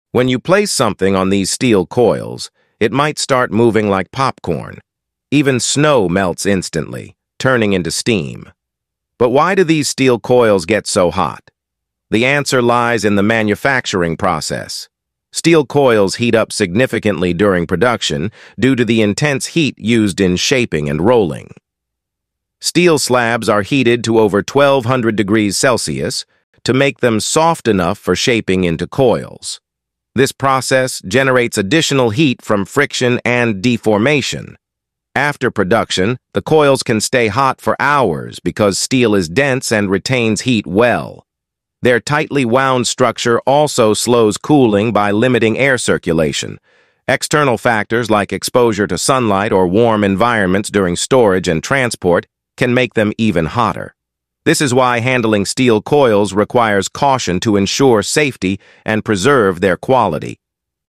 How Dangerous Steel Coils Are 😱
Steel coils may look harmless, but did you know they can weigh several tons and pose serious risks during transport? Discover the shocking facts about these heavy-duty hot rolled steel coils, including how they’re made, what they’re used for, and why they require special handling during steel coil transport.

In this video, The Upbeat Zone dives into the fascinating world of steel coils—covering everything from how long a steel coil is to the unique technology behind their production. Whether you’re curious about hot rolled steel coil facts, or want to learn why steel coil transport is considered one of the most challenging aspects of heavy transport, this video is packed with science facts and interesting facts you don’t want to miss!

Don’t forget to check out our viral shorts, yt shorts, and other fact videos on technology and unique innovations.